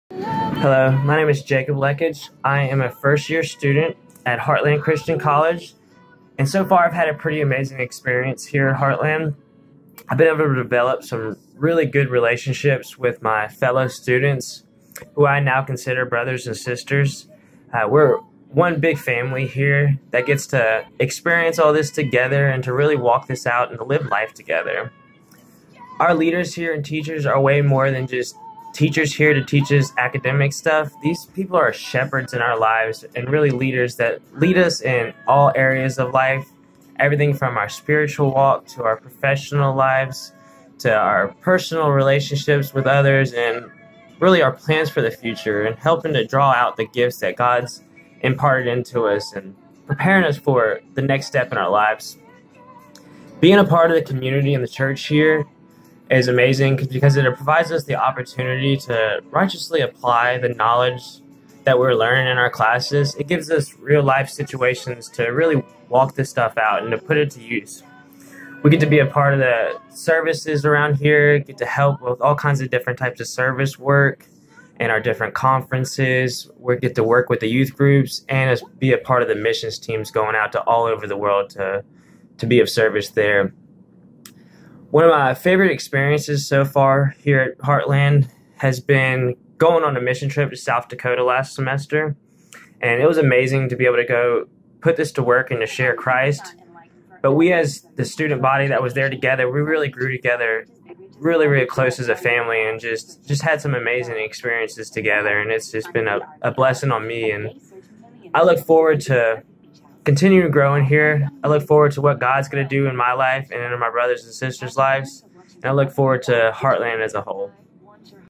Jacob Leckich - Student
Jacob is one of our current students and will be graduating in the Class of 2019. He recently graduated from the Heartland Men's Recovery Program. He currently works as a Safety manager for CNS Ministries. In his spare time, he also serves in Heartland Community Church's youth group.